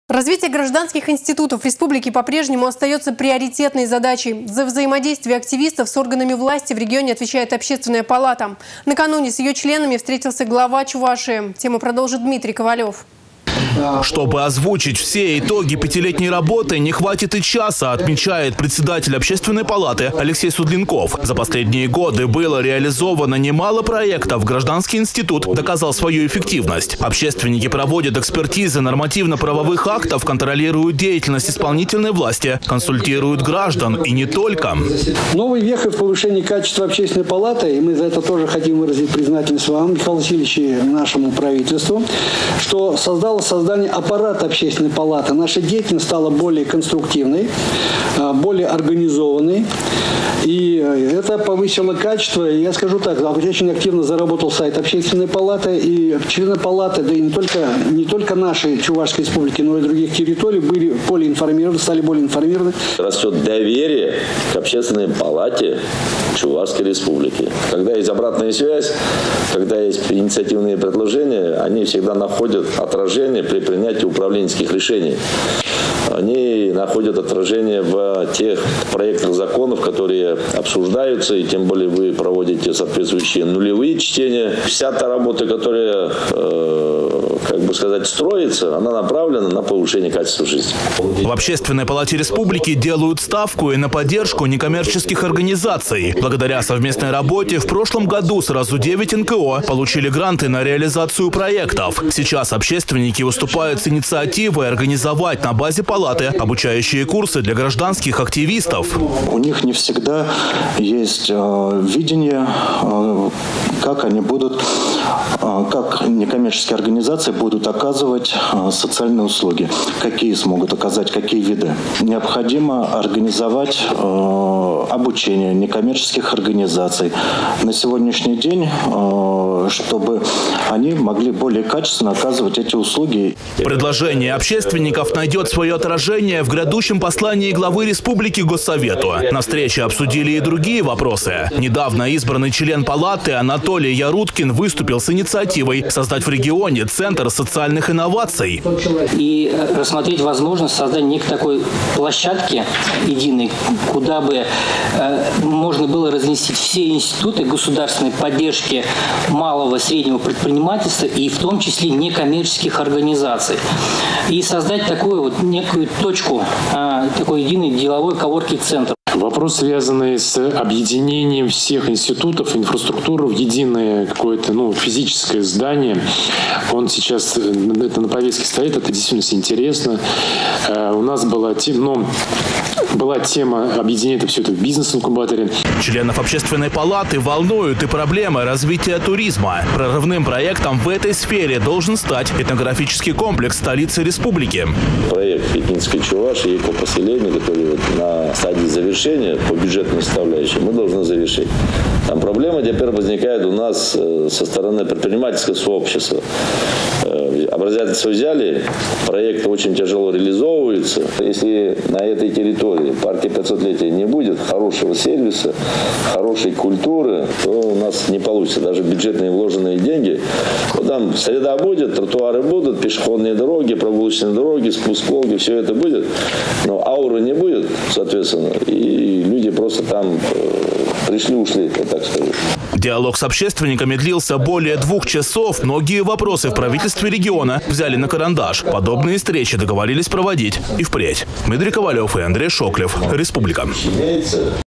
Глава Чувашии Михаил Игнатьев встретился с членами Общественной палаты Чувашской Республики.
Развитие гражданских институтов в республике по прежнему остаётся приоритетной задачей. За взаимодействие активистов с органами власти в регионе отвечает Общественная палата. Накануне с её членами встретился Глава Чувашии.